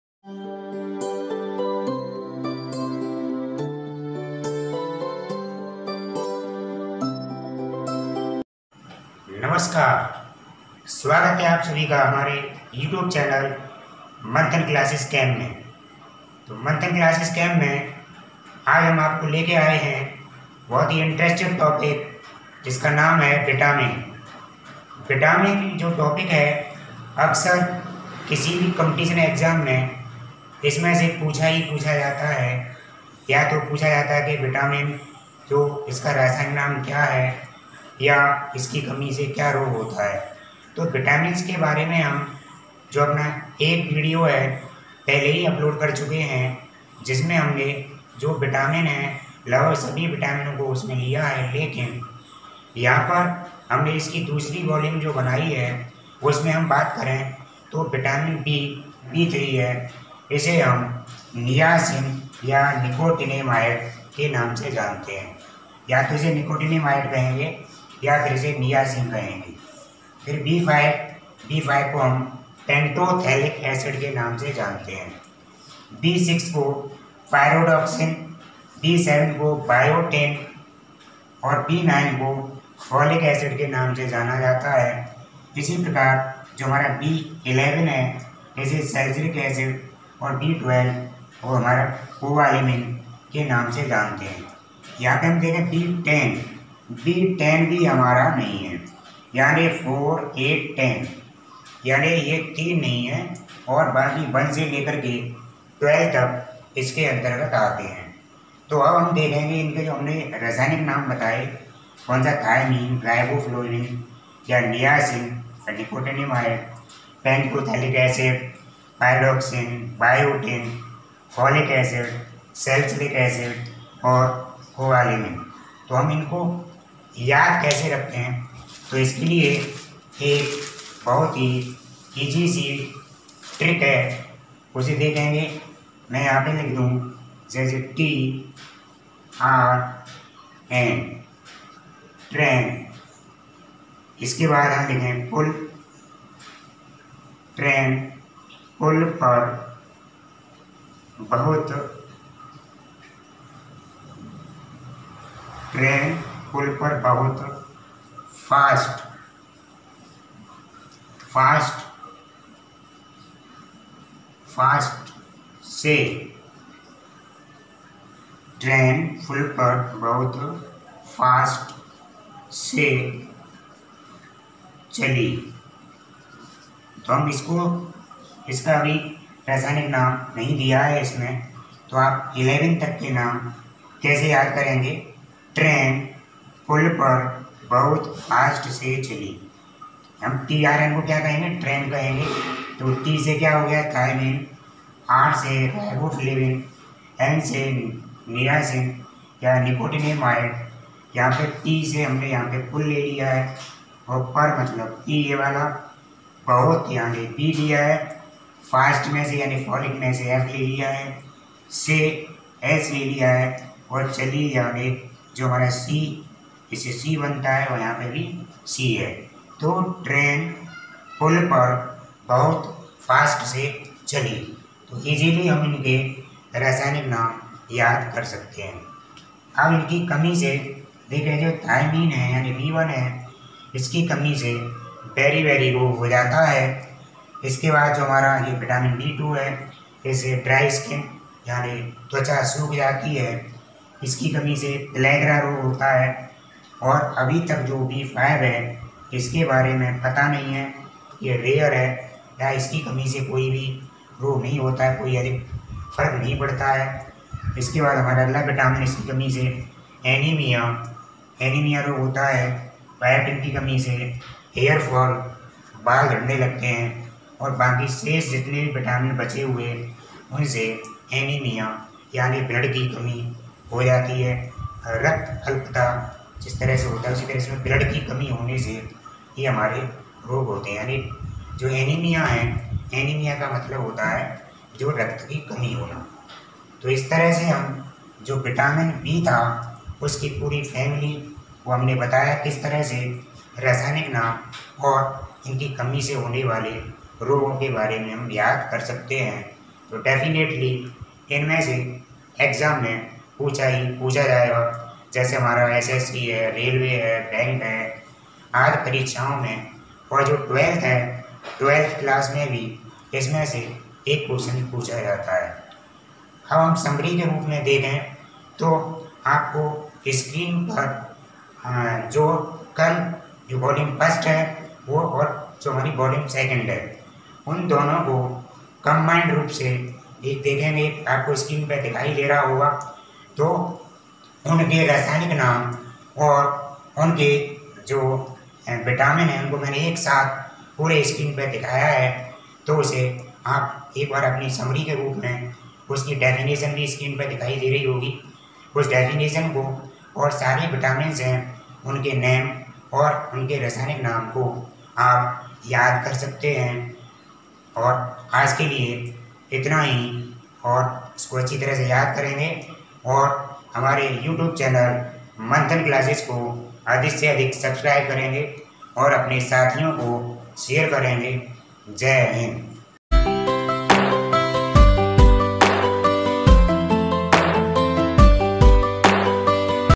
vitamin Vol. 02(विटामिन 'B' Family)
इस vedio के माध्यम से आप विटामिन B के सभी सदस्यों के रासायनिक नाम आसानी से याद कर सकते है